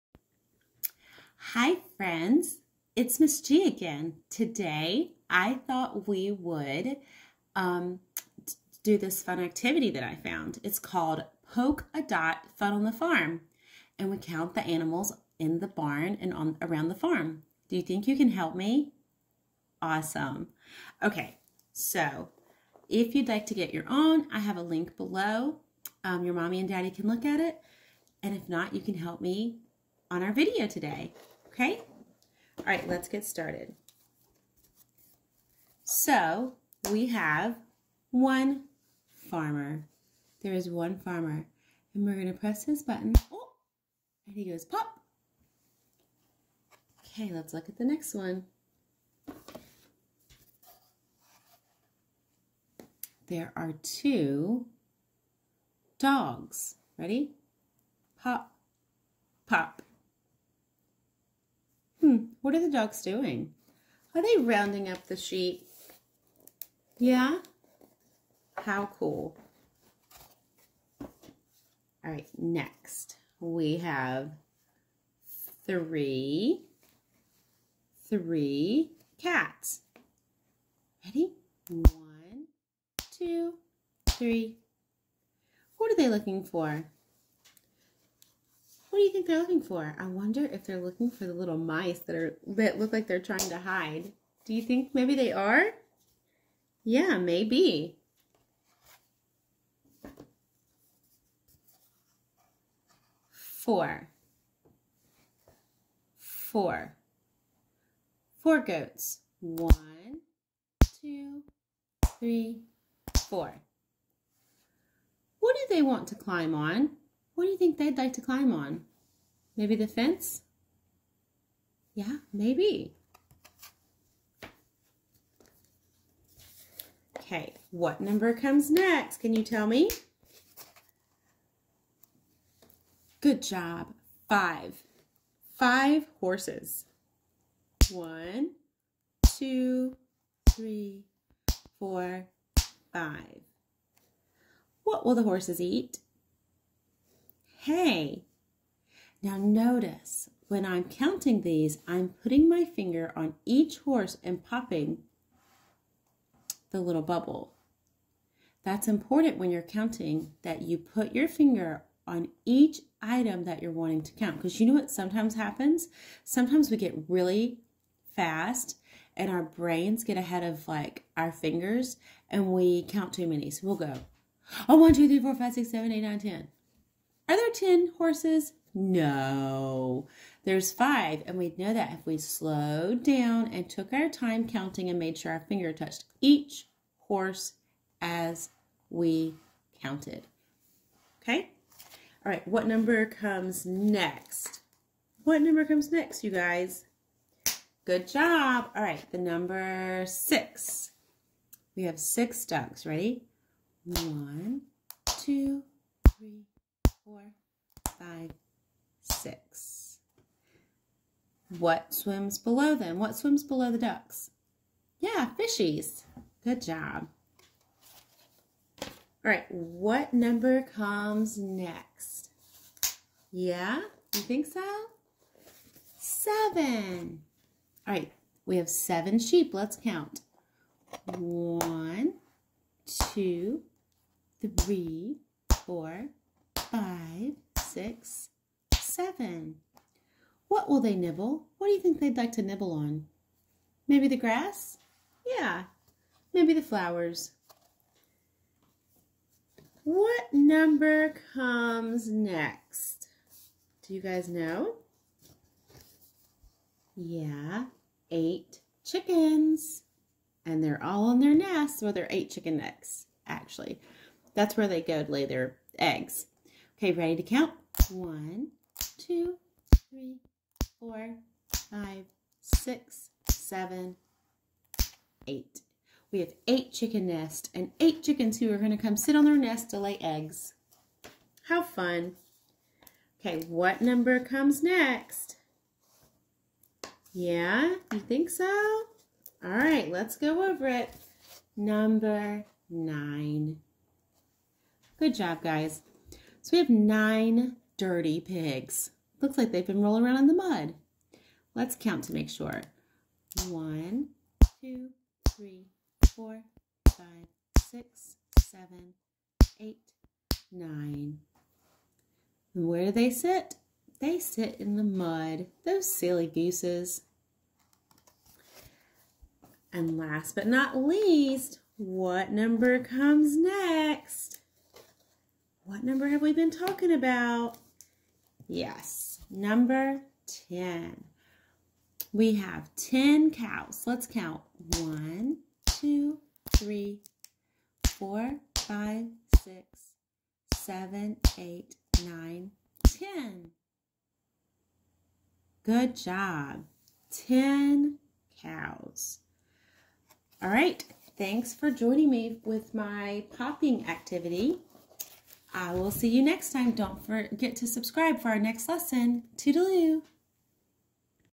Mrs. G's Circle Time: KReady Counting to 10 using Poke a Dot
In this video, we will count numbers 0-10  using our Poke a Dot activity.

Poke a Dot Farm
Poke a Dot 10 Little Monkeys
Poke a Dot Number Cards

 These are great for kinesthetic learners!